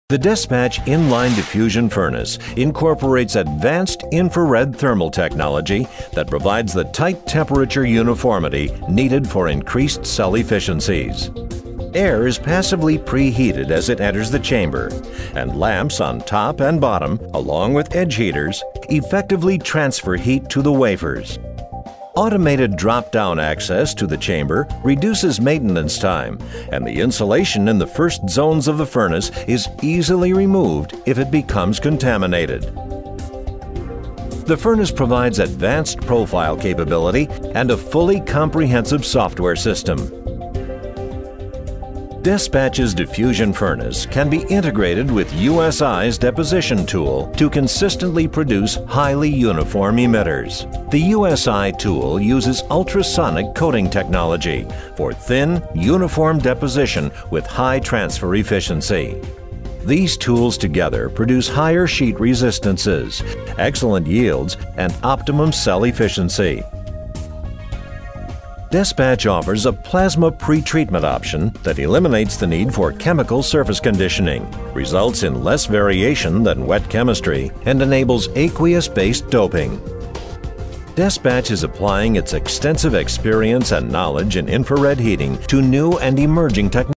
Despatch In-Line Diffusion Furnace
The Despatch In-line Diffusion Furnace incorporates advanced infrared thermal technology that provides the tight temperature uniformity needed for increased cell efficiencies. Air is passively preheated as it enters the chamber and lamps on top and bottom along with edge heaters effectively transfer heat to the wafers. Automated drop-down access to the chamber reduces maintenance time and the insulation in the first zones of the furnace is easily removed if it becomes contaminated. The furnace provides advanced profile capability and a fully comprehensive software system.